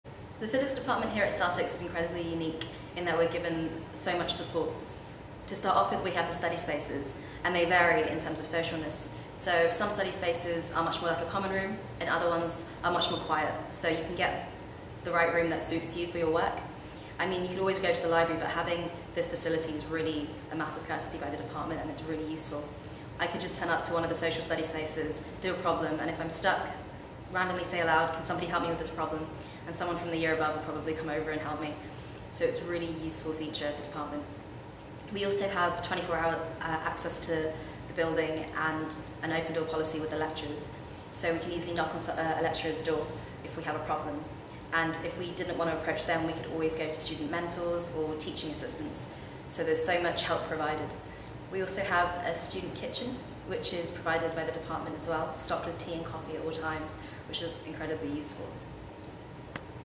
MPhys Theoretical Physics student Anjelah
Undergraduate MPhys Theoretical Physics student Anjelah talks about the unique environment and support within the Department of Physics and Astronomy at the University of Sussex